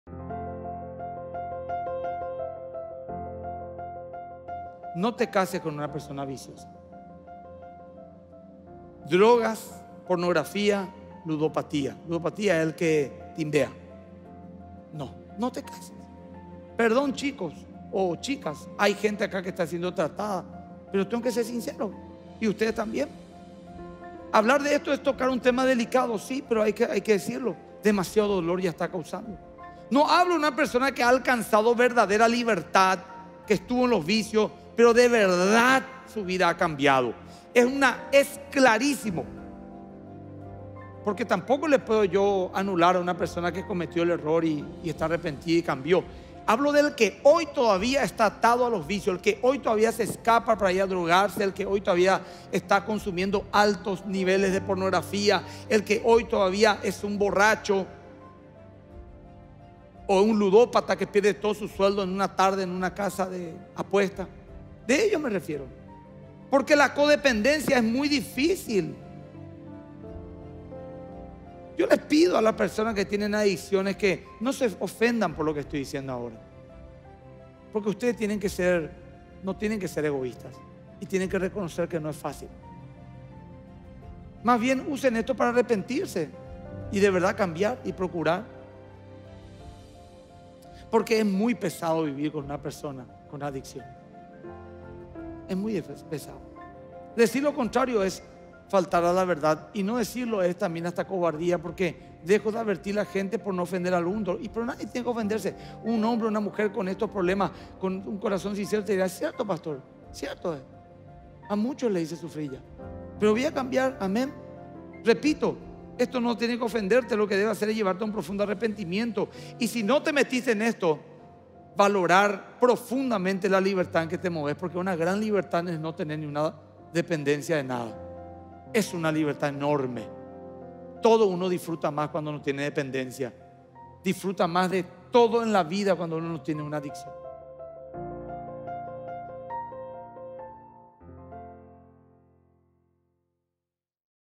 EnPP005 En pocas palabras | No te cases con una persona viciosa
Escuchá hoy el consejo y determiná no involucrarte sentimentamente ni casarte con una persona que aún está luchando con algún tipo de adicción. ¡Te ahorrarás muchos dolores y aflicciones futuras! ¡Sé sabio/a!

➡️ Si estás luchando con alguna adicción o conocés a alguien cercano que está pasando por esto, te invitamos a participar de nuestras reuniones de apoyo Perseverantes, todos los martes a las 19:30, en Más que vencedores.